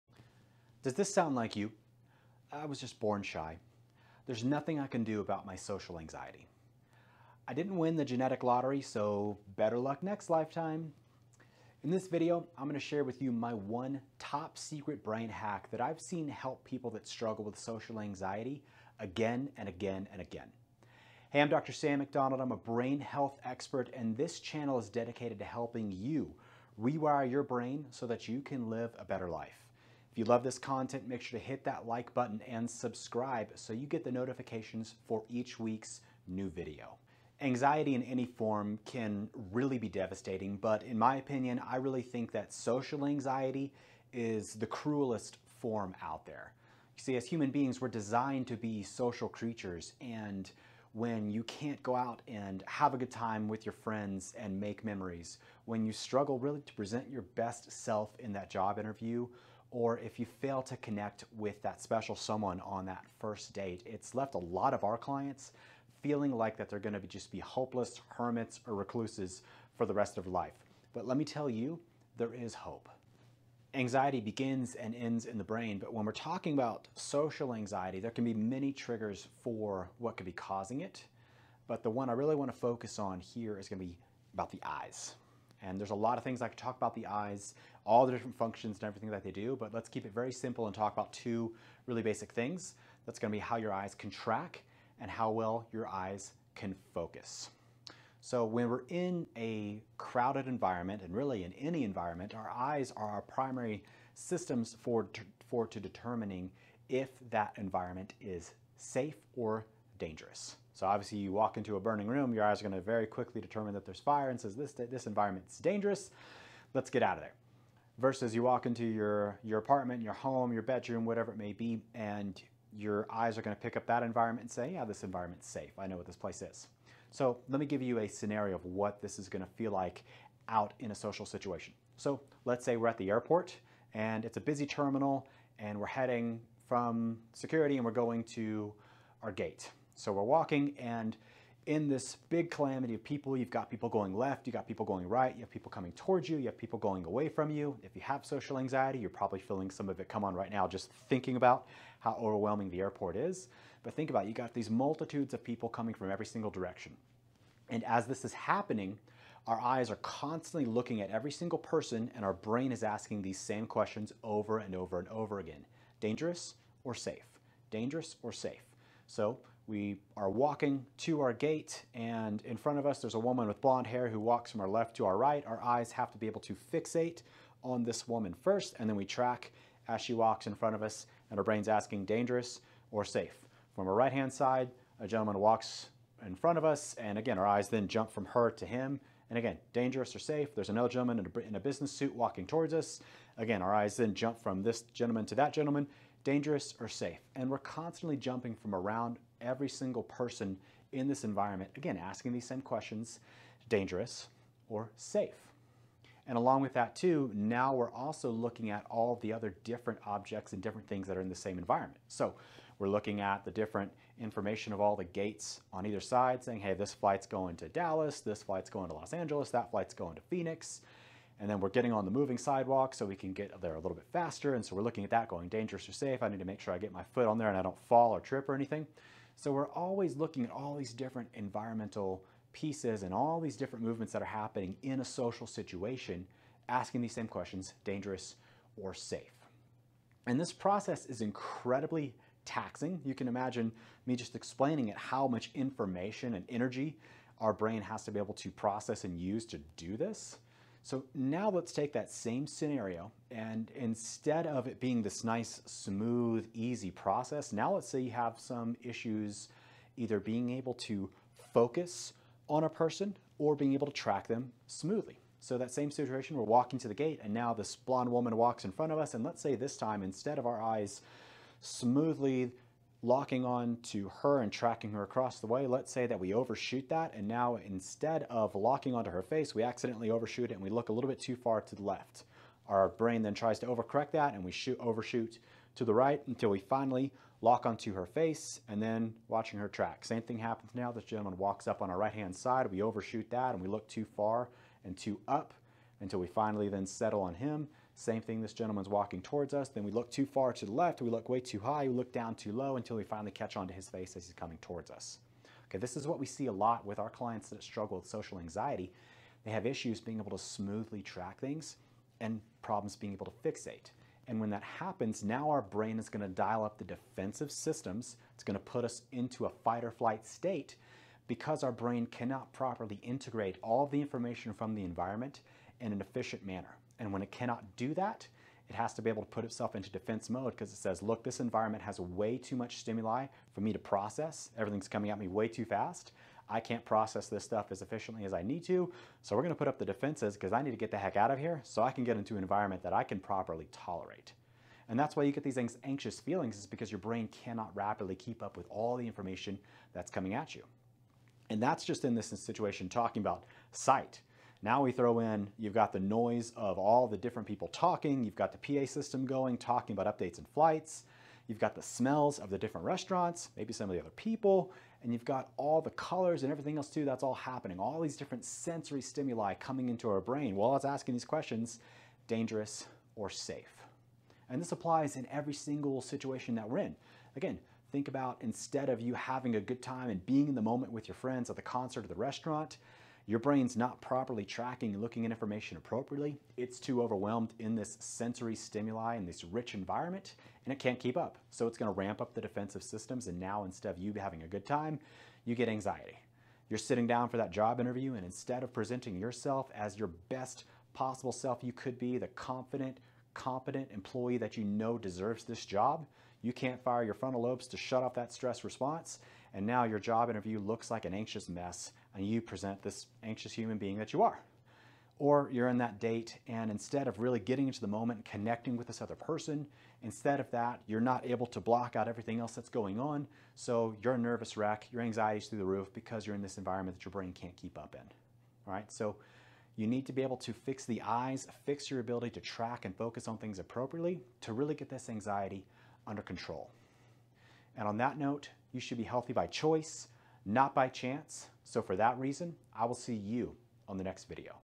How To Beat Social Anxiety | Denver Brain Health Expert Explains
In this video Denver Brain Health Expert shares a secret Brain hack to overcome social anxiety

watch this video and learn my brain health tips!

TIME STAMP
0:00 Intro to Social Anxiety video
0:37 Social Anxiety is the worst
1:13 Anxiety Begins in the Brain
1:39 The Eyes and Anxiety
4:26 The Eyes not working and Social Anxiety